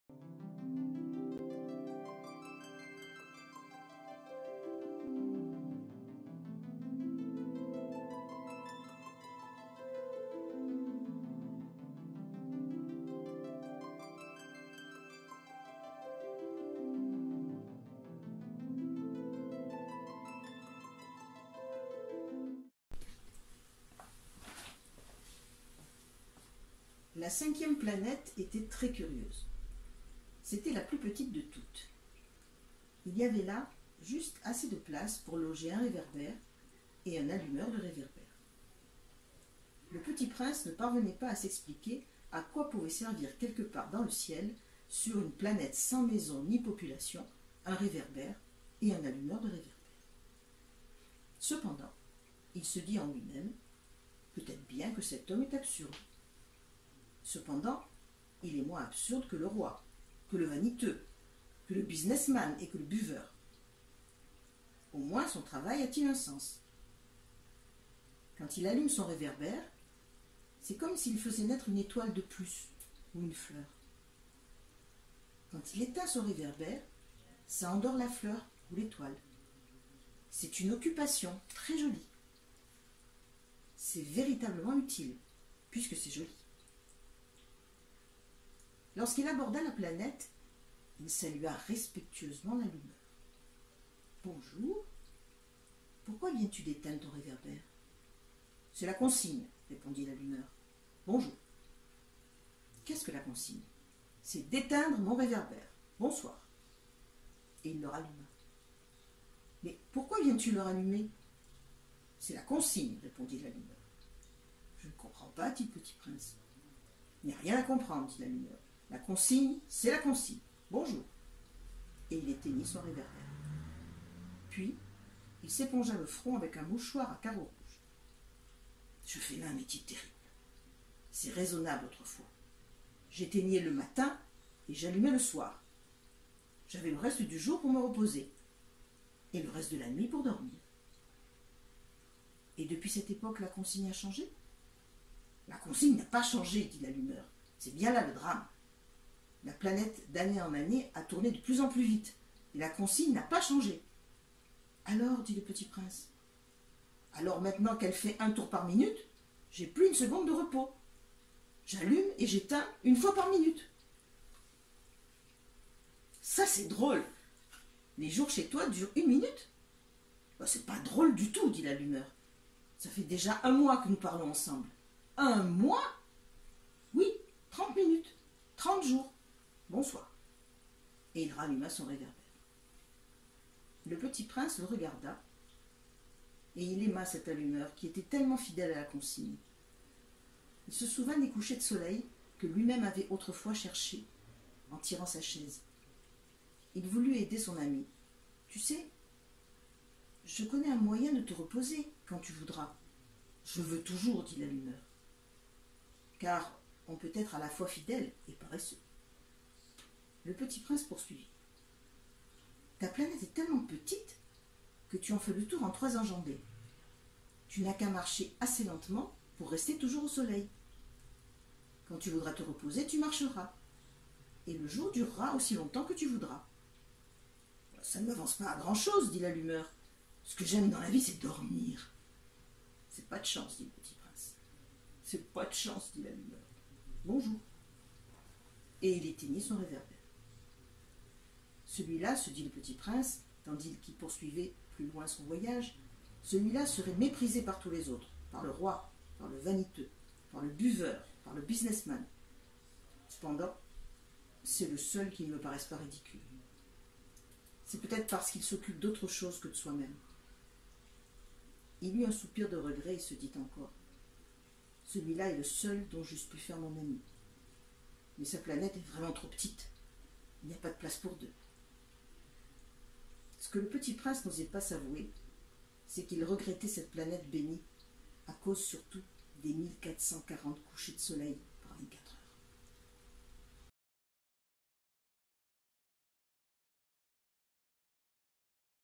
Le Petit Prince - Chapitre 14 - Madeleine - Lecture Vidéo
Durant le confinement dû à la pandémie actuelle, la Fondation Antoine de Saint-Exupéry pour la Jeunesse et l'Association Arrimage, vous proposent de vous évader avec le Petit Prince!

Nous avons invité  tous nos amis, notamment les mal et non-voyants, à nous rejoindre dans cette initiative et à partager avec nous, leurs extraits du conte le plus traduit au monde!

Voici une des  vidéos de Madeleine  qui a choisi le chapitre Chapitre 14

Merci à  elle